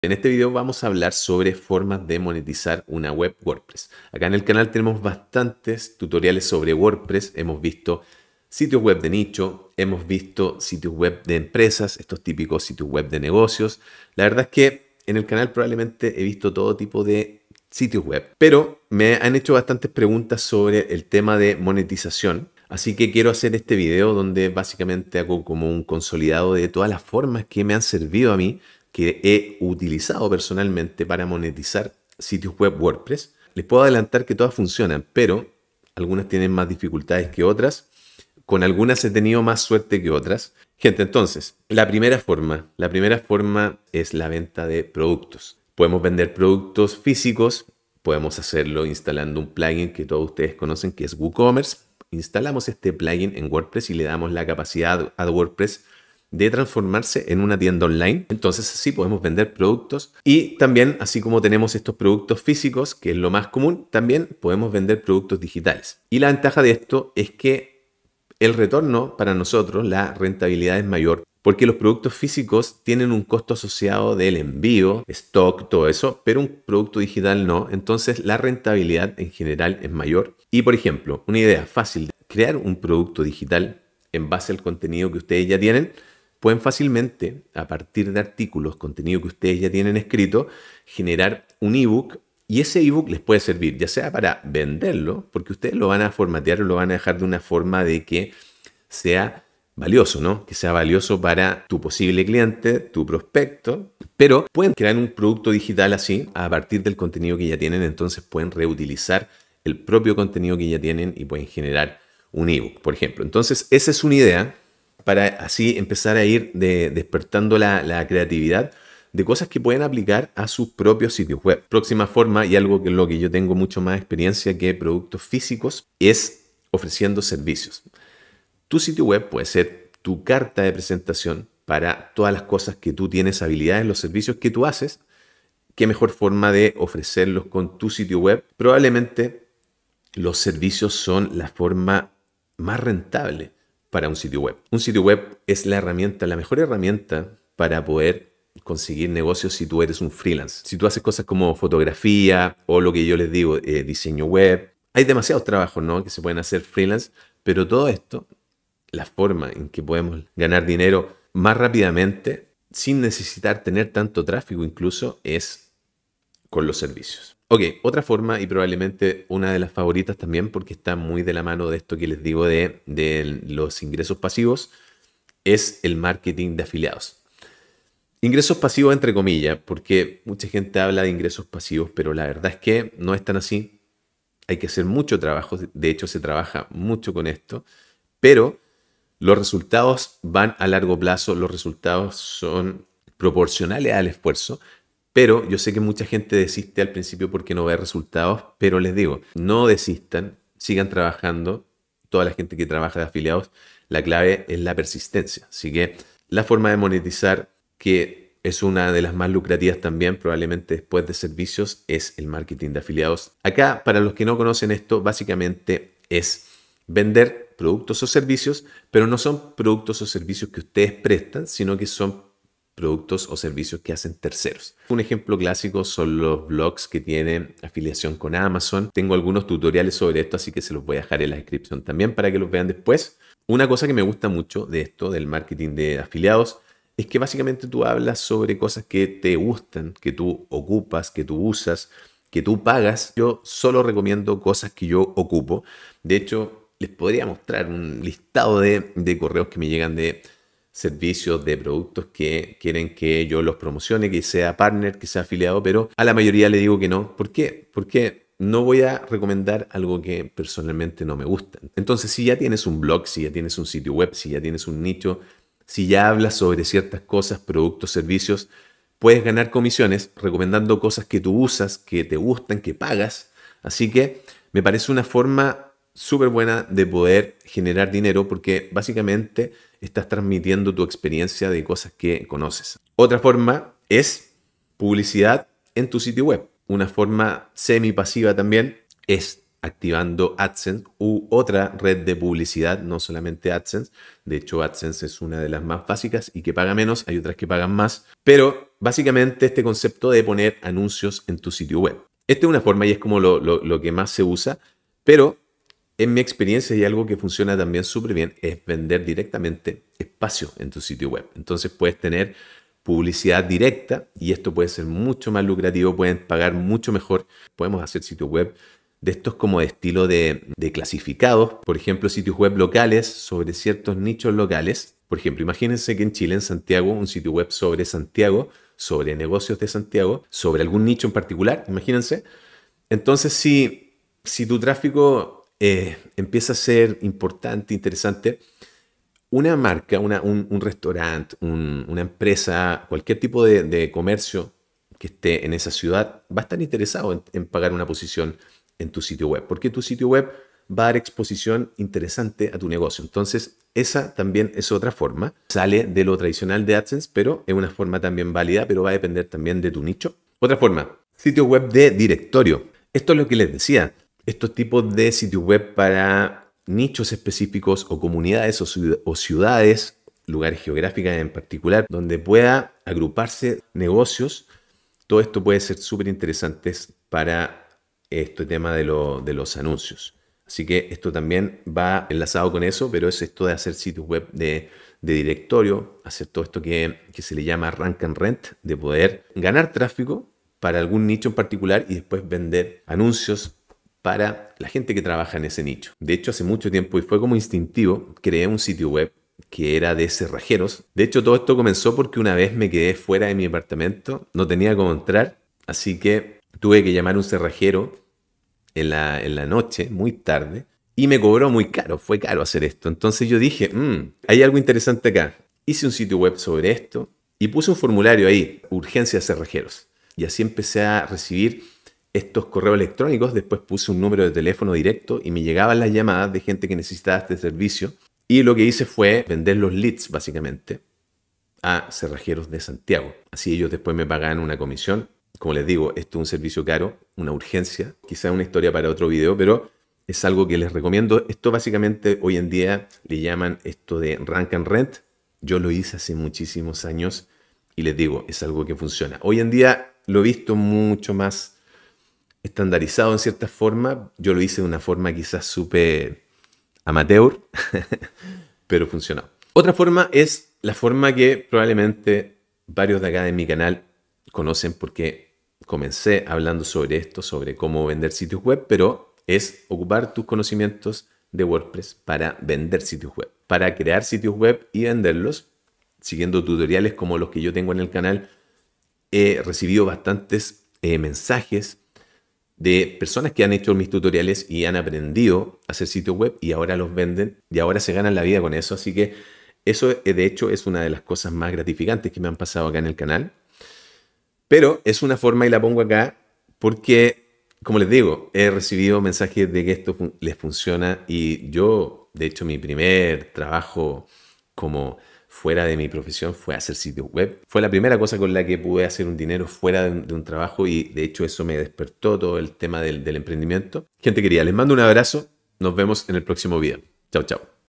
7 Formas Probadas de Monetizar una Web WordPress
estas son las formas en las que tengo experiencia monetizando webs WordPress, tengo varios tutoriales sobre cómo crear este tipo de sitios web en el canal

Usa este enlace para obtener el mejor precio en Hostinger